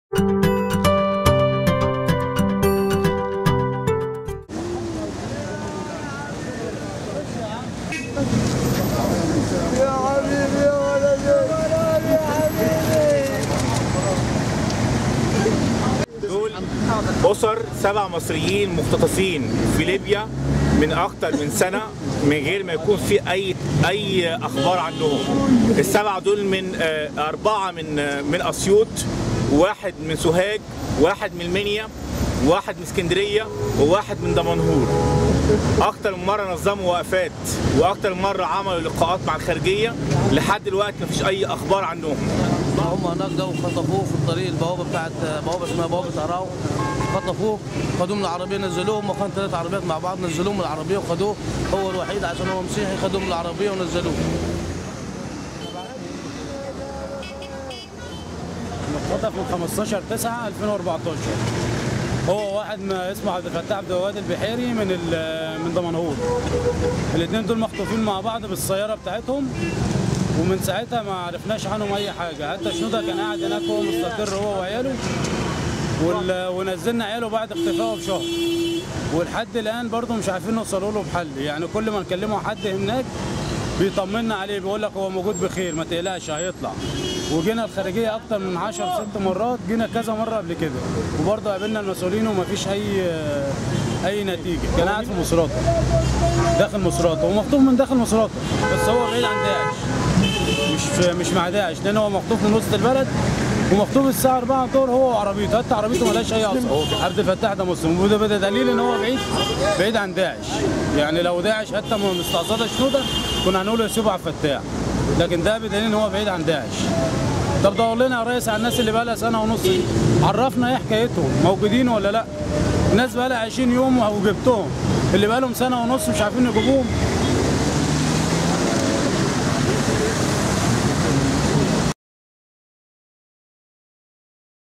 شقيق أحد المختطفين بليبيا للسيسي: «اللي بقالهم ٢٠ يوم رجعوا و اللي بقالهم سنة ونص مرجعوش»
تقرير: عمر ساهر